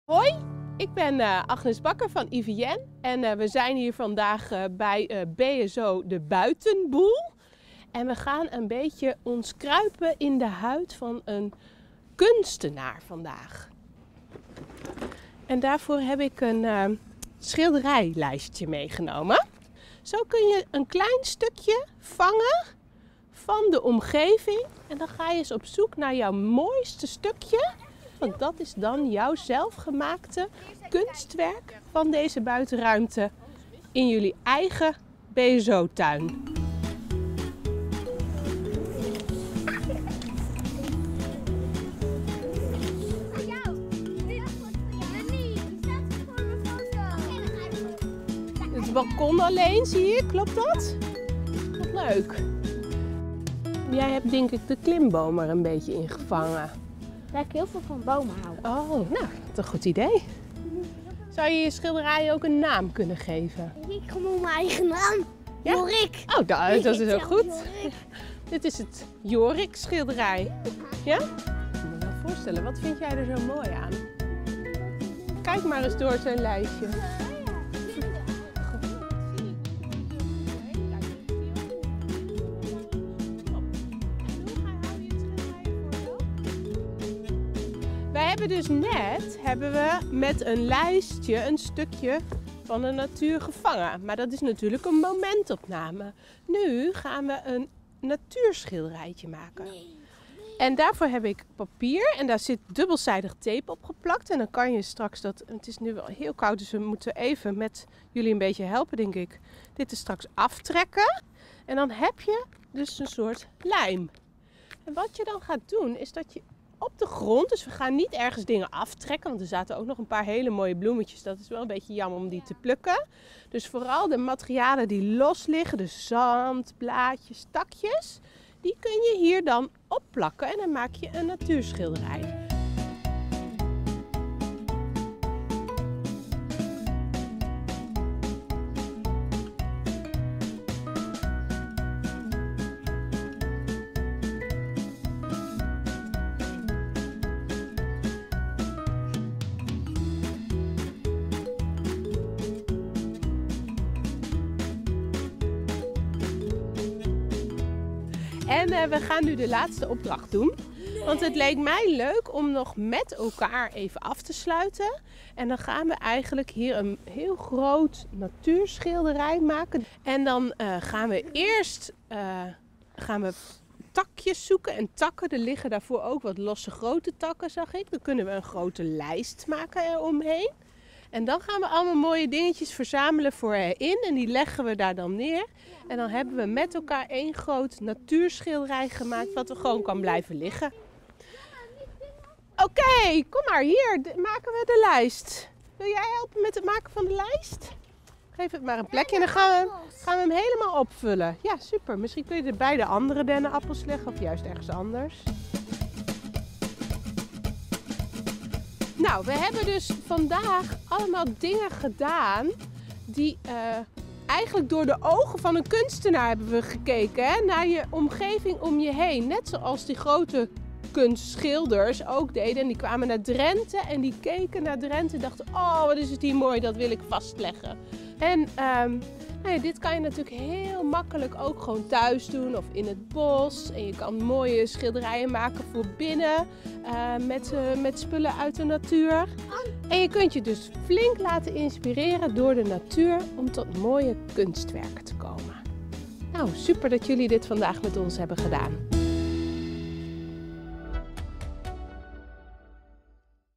ROEG! Junior | Op pad met IVN: natuurschilderijtjes maken
Kinderen van BSO De Buitenboel in Gieten kruipen in de huid van een kunstenaar. Samen met Agnes Bakker van IVN Natuureducatie maken zij natuurschilderijtjes.

Wil je meer weten over de Drentse natuur? Volg ons dan ook via de ROEG! socials.